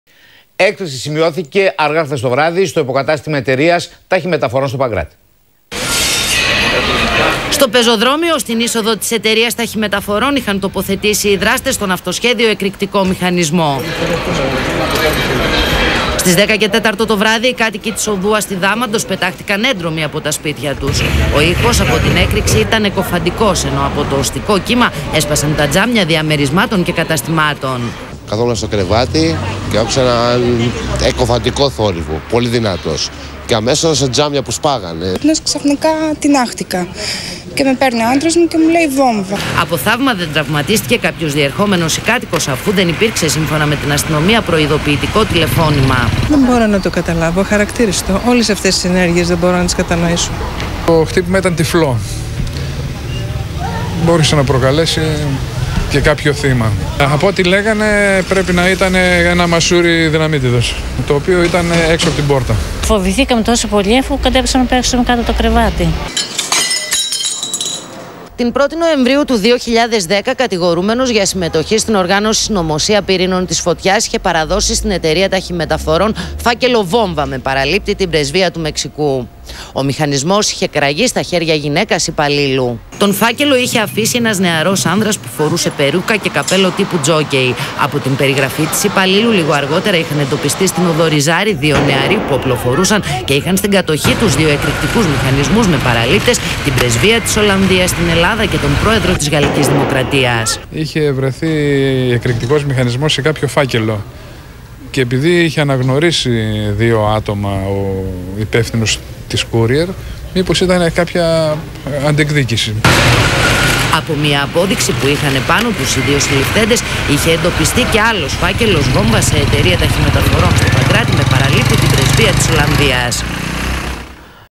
11.3.13-Έκρηξη σε εταιρεία ταχυμεταφορών στο Παγκράτι
Μεσημεριανό δελτίο ειδήσεων - Δευτέρα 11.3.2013 - με τον Θοδωρή Δρακάκη.
"Έκρηξη σε εταιρεία ταχυμεταφορών στο Παγκράτι"